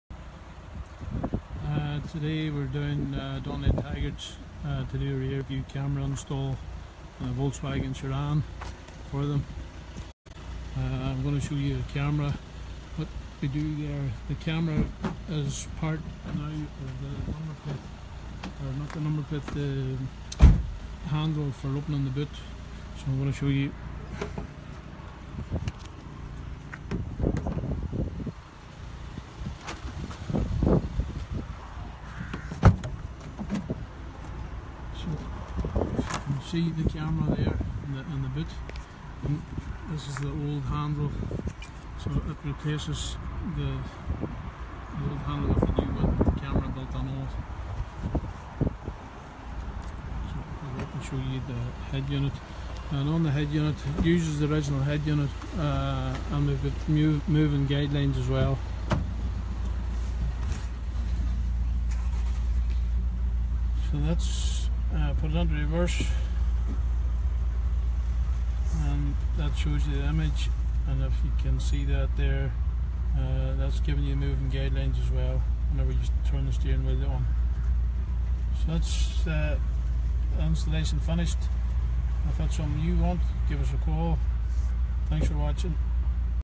VW Sharan Rear View Camera install
Here we installed a Rear View Camera for reversing to this brand new VW Sharan. The camera comes built into a new grab handle for the boot and it is integrated through the screen in the dash with moving guidelines.